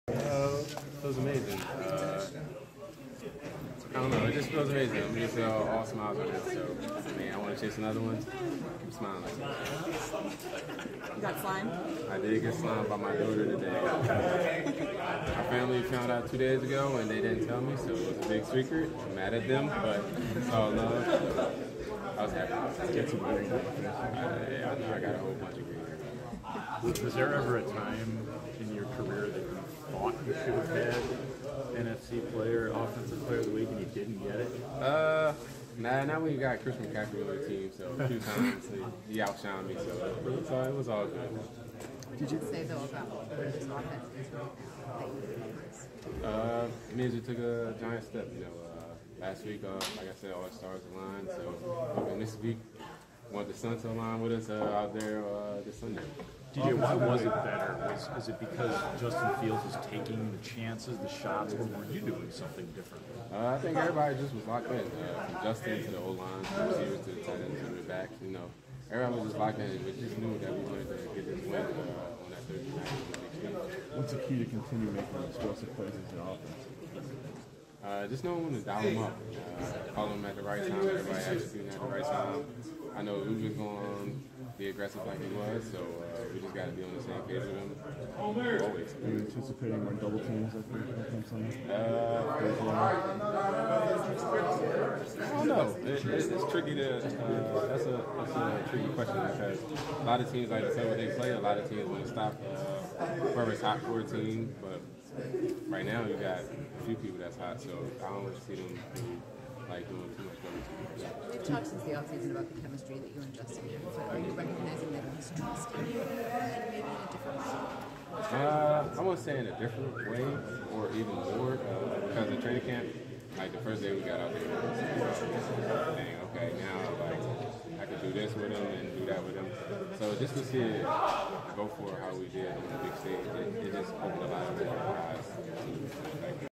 Bears WR DJ Moore - 10.11.23
DJ Moore discusses winning NVP and NFC Offensive Player of the Week in Week 5.